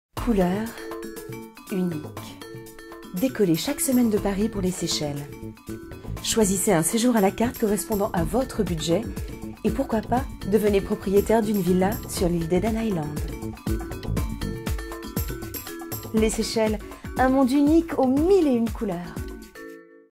Office du tourisme des Seychelles - Spot TV de Guillaume Didier et Richard Martineau
Spot TV diffusé sur LCI et TEVA - Réalisé par Guillaume Didier et Richard Martineau - Production Exécutive : Richard Martineau